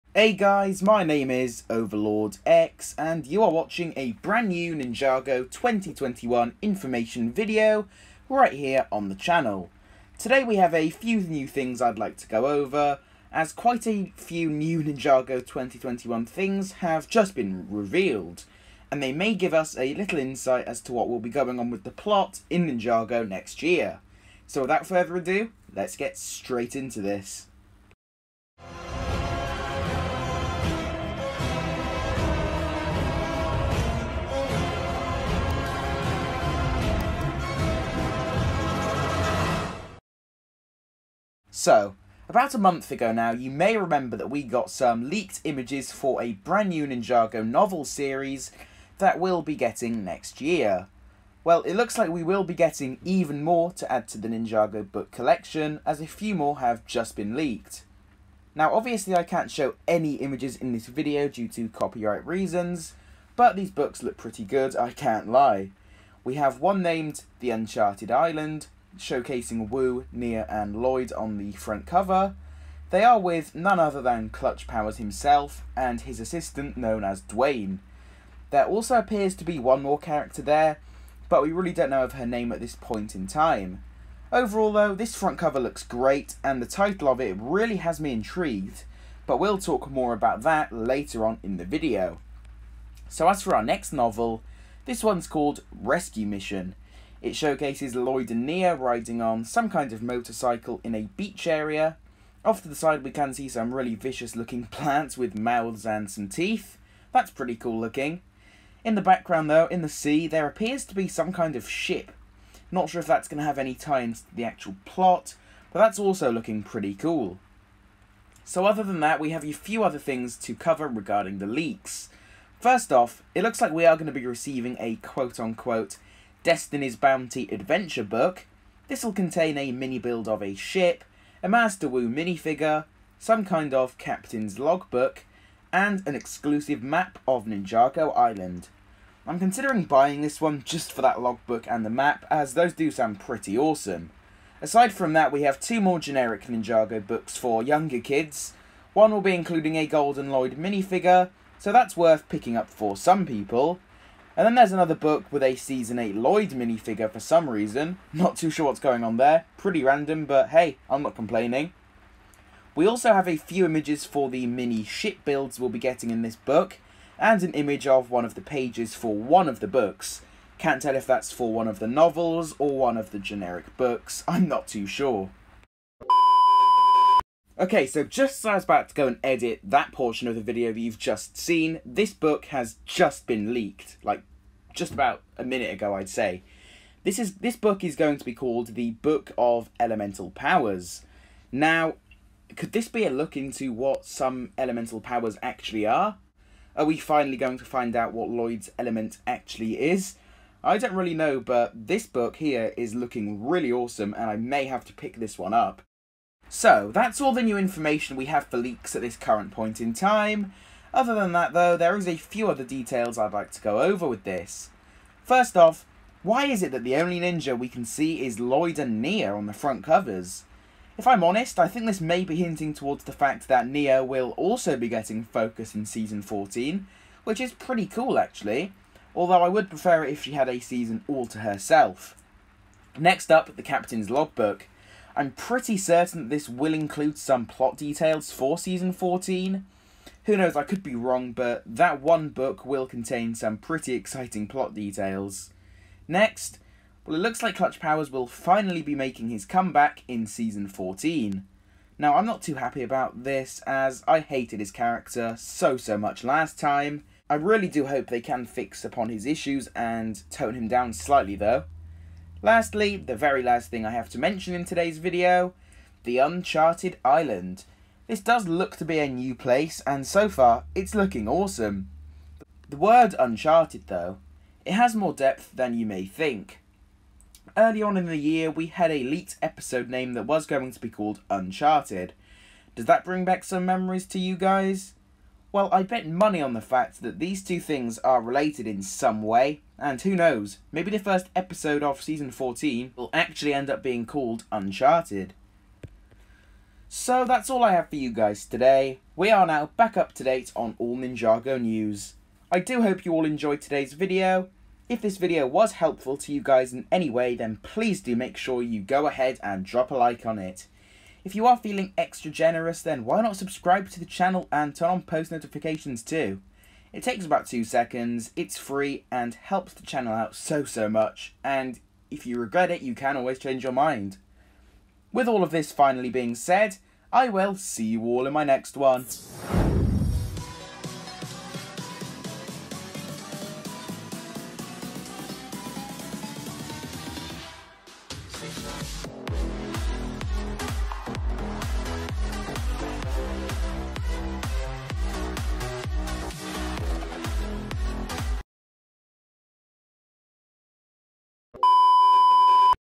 Ninjago Season 14/Legacy: NEW Books Revealed! + New Plot Details!?
Copyright

[Credit]
Video Provided By : NoCopyrightVideoHD
[NoCopyrightVideoHD]